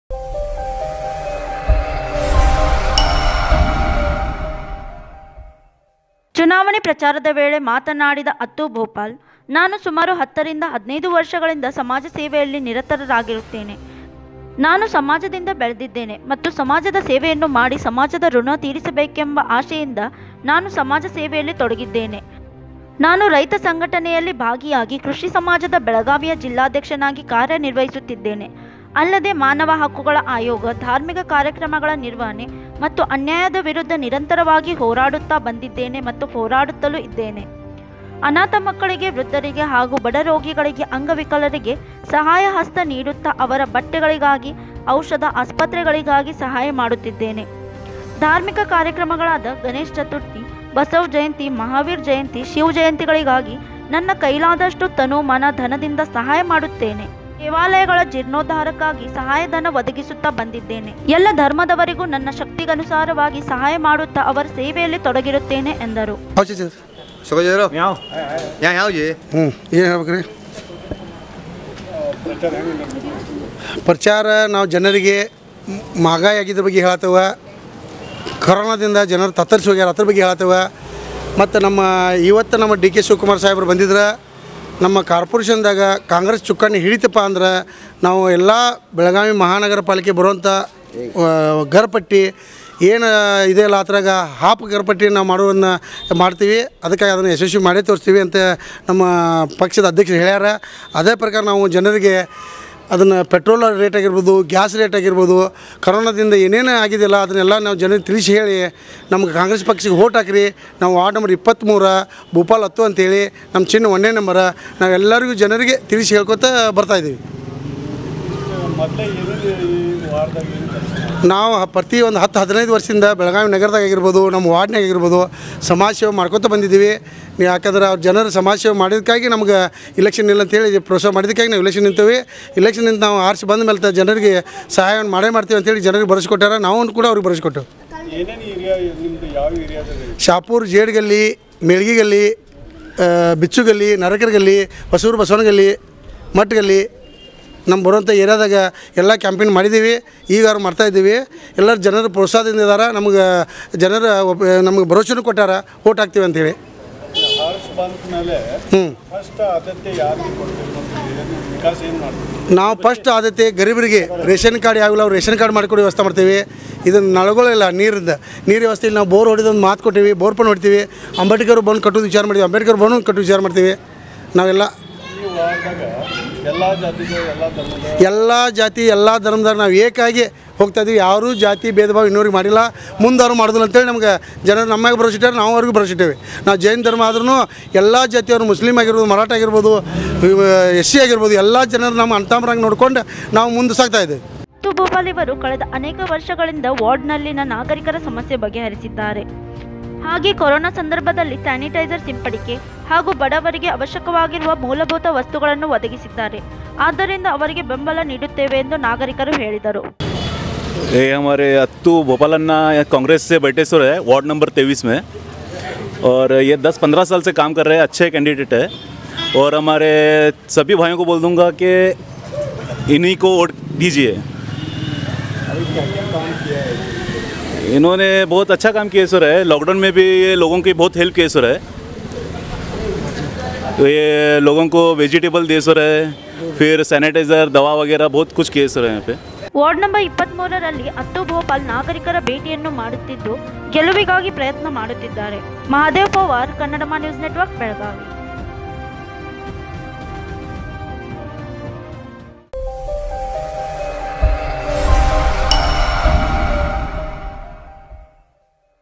Attu Bhupal Campaigning For Upcoming Elections Getting Huge Support From Citizens Of Ward No. 23
ಚುನಾವಣೆ ಪ್ರಚಾರವನ್ನು ನಡೆಸುತ್ತಿದ್ದ ಅತ್ತು ಭೂಪಾಲ
೧೦-೧೫ ವರ್ಷದಿಂದ ಸಮಾಜ ಸೇವೆಯಲ್ಲಿ ತೊಡಗಿದ್ದ ಭೂಪಾಲ
ಸಮಾಜದ ಋಣ ತೀರಿಸಬೇಕೆಂಬ ಆಸೆ ವ್ಯಕ್ತ ಪಡಿಸಿದ ಭೂಪಾಲ
ಬೆಳಗಾವಿ ಜಿಲ್ಲಾಧ್ಯಕ್ಷನಾಗಿ ಕಾರ್ಯ ನಿರ್ವಹಿಸಿದ್ದೆನೆ
ಧಾರ್ಮಿಕ ಕಾರ್ಯಕ್ರಮಗಳಿಗೆ ಸಹಾಯ ನಿಡುತ್ತೆನೆ
ಅನ್ಯಾಯದ ವಿರುದ್ದ ಹೊರಾಡುತ್ತ ಬಂದಿರುವ ಭೂಪಾಲ ಅತ್ತು
ಅಧಿಕೃತ ಬೆಂಬಲಿಗರನ್ನು ಪಡೆದ ಅತ್ತು ಭೂಪಾಲ